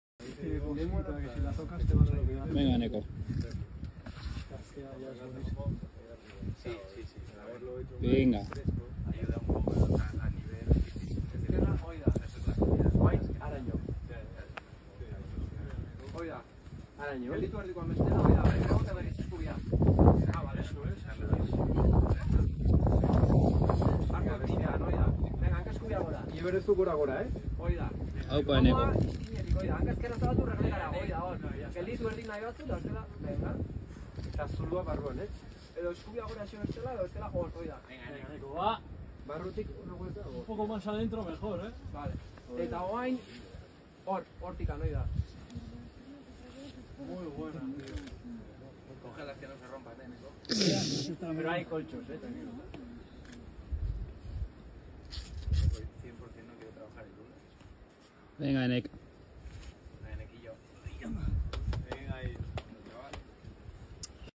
El de la campa, 7c+/8a (sit). Larraona
Climber: Eneko Carretero
Name: El de la campa
Grade: 7c+/8a
Zone: Larraona